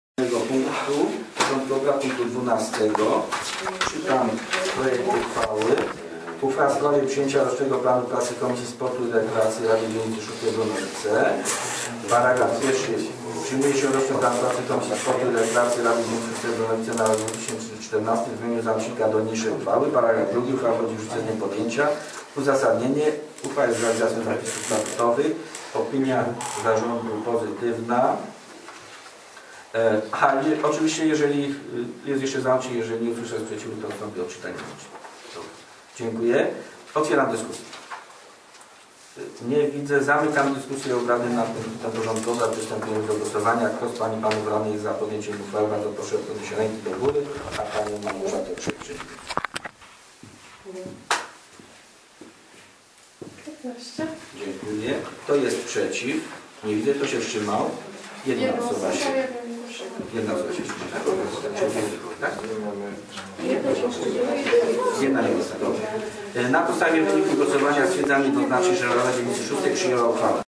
RD_VI_13-02-2014_12 przyjęcie rocznego planu pracy Komisji Sportu i Rekreacji
Obrady XXXIX sesji Rady Dzielnicy VI Bronowice z dn. 13 lutego 2014 r. Uchwała w sprawie przyjęcia rocznego planu pracy Komisji Sportu i Rekreacji.